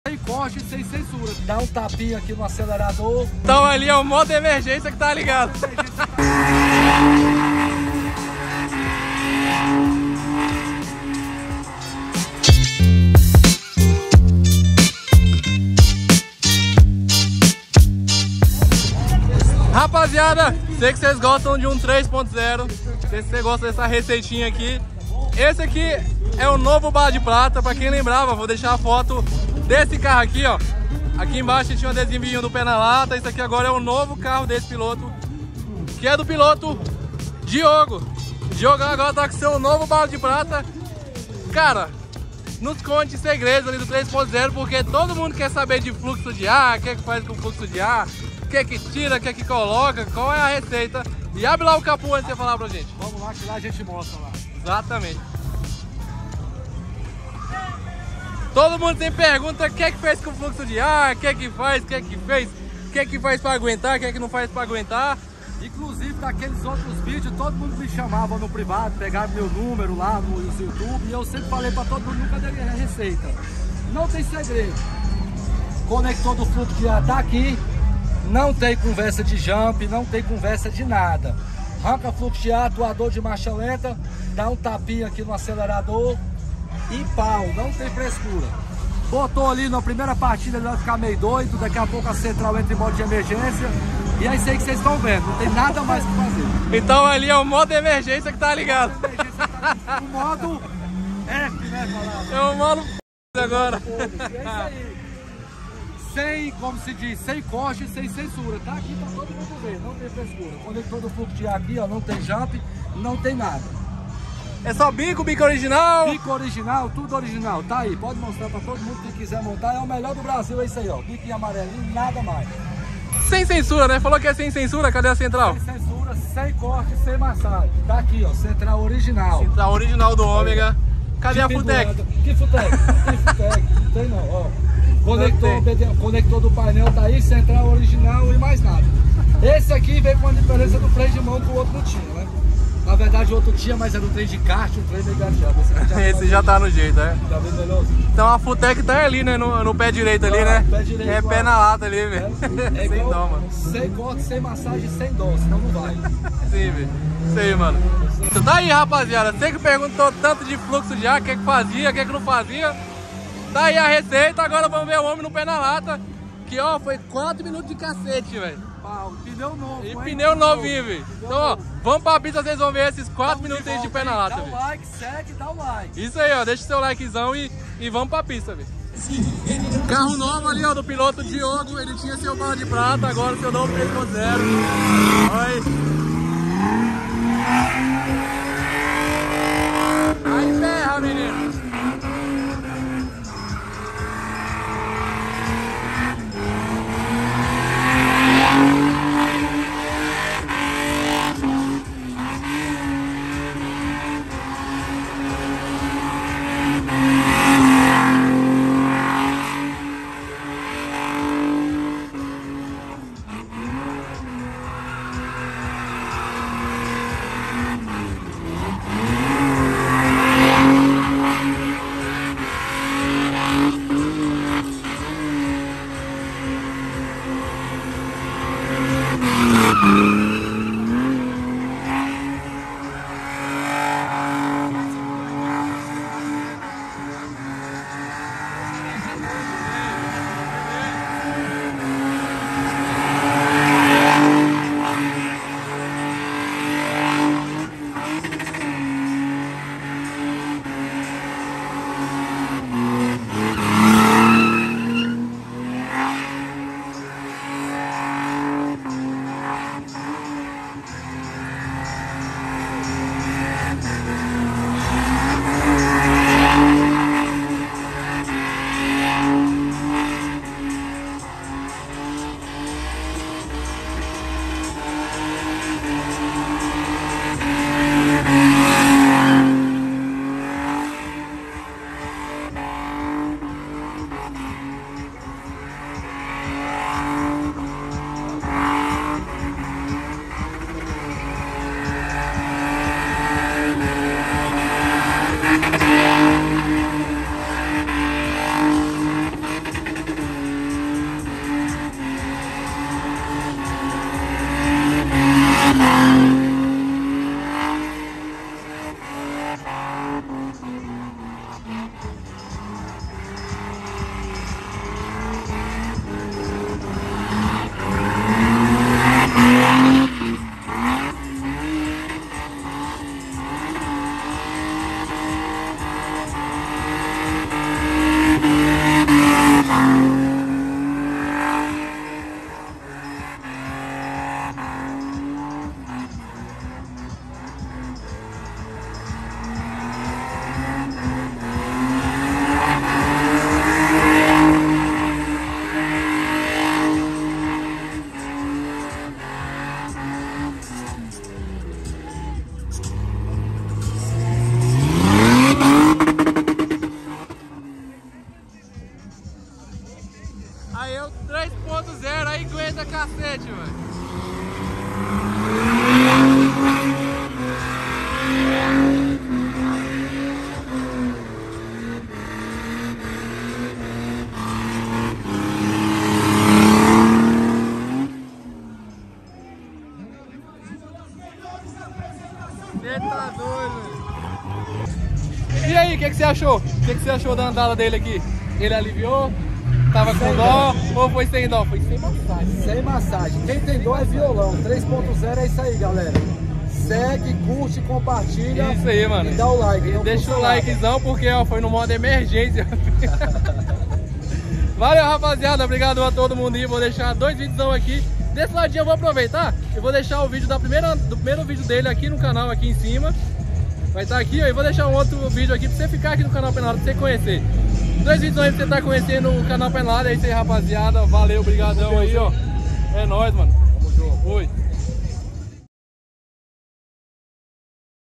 MODO EMERGÊNCIA ATIVADO! OMEGA 3.0 ORIGINAL // Canal Pé Na Lata
Ative o sino, cara! 🔔
Apoie o canal, seja um pé na lata!

No vídeo, o piloto Diogo apresenta seu novo Bala de Prata 3.0 original. Sem cortes e sem censura, ele compartilha os ajustes feitos, destacando a originalidade dos componentes. Após as modificações, o carro é levado para a pista para um teste emocionante. Diogo convida os espectadores a deixarem seus likes e compartilharem o vídeo. Ele também sugere outros vídeos relacionados. Agradece pela audiência e promete mais conteúdo emocionante no canal. BarDePrata 3PontoZero SemCortes SemCensura DiogoNoVolante.

Este canal é voltado para preparação de carros,

Apresentação, produção e edição:
Leandro Lima